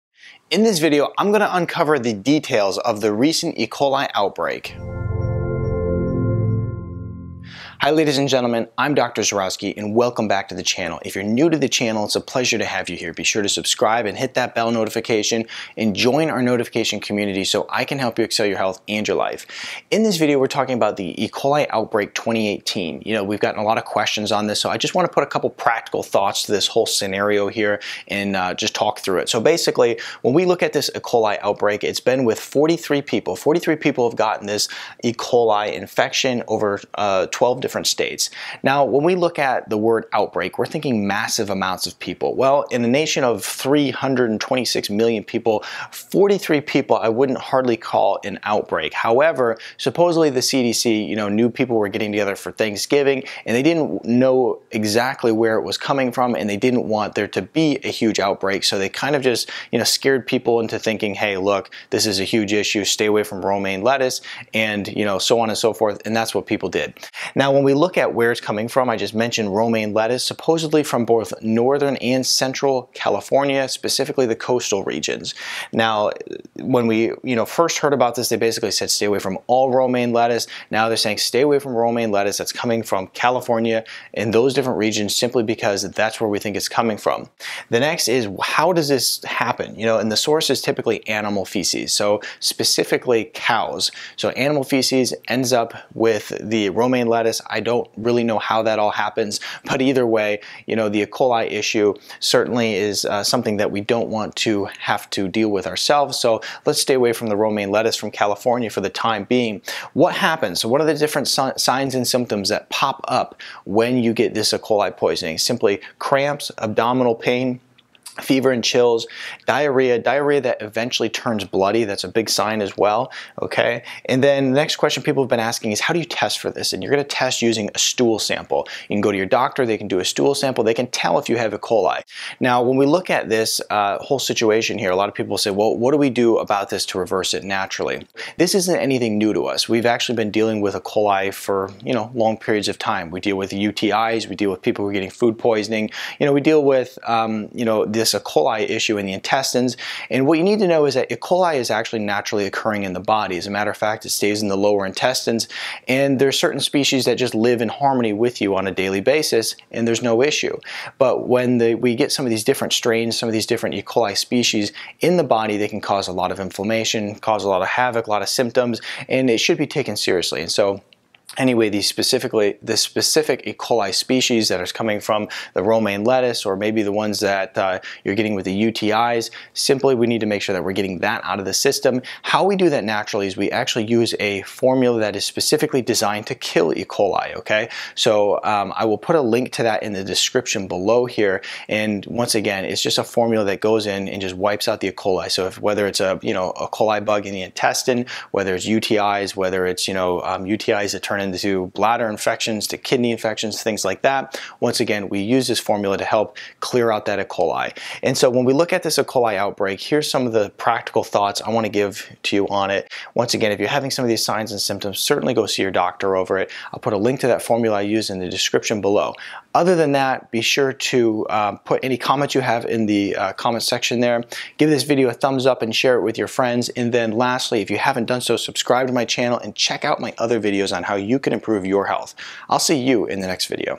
E Coli Outbreaks: What You NEED To KNOW | Dr. Nick Z.
E. Coli Outbreak 2018 | What You Must Know is a video putting some practical thoughts to the recent E. Coli outbreak.

The romaine lettuce E. Coli outbreak is an interesting topic as there were very few people that were ever infected and yet it was considered an outbreak. Supposedly the CDC wanted to spread the word before people gathered around for Thanksgiving dinner potentially spreading the bacteria to a high level.

The E. Coli bacteria typically comes from animal feces, typically cows. It is very commonly found in meats but can also even be found, in this case, romaine lettuce. It is important to know that E. Coli is a naturally occurring bacteria in the lower intestines that is there in particular amounts comprised of particular species. The E. Coli romaine lettuce is a species of E. Coli that you don't want in your gut because it will cause many symptoms.

E. Coli symptoms include, but are not limited too, diarrhea that turns bloody, fever and chills, cramps, abdominal pain, and lastly inflammation. If you have any of these symptoms and you have eaten romaine lettuce, then you should go see your doctor and they can test for E. Coli using a stool sample.

E. Coli outbreak 2018 is obviously serious if your someone who has eaten lettuce and now have all the symptoms. But it certainly seems far from an outbreak in a nation of 326 million people, when only 42 people have contracted it at the time of me writing this post.

48864 Romeo Plank
Macomb Mi, 48044
Dr. Nick Zyrowski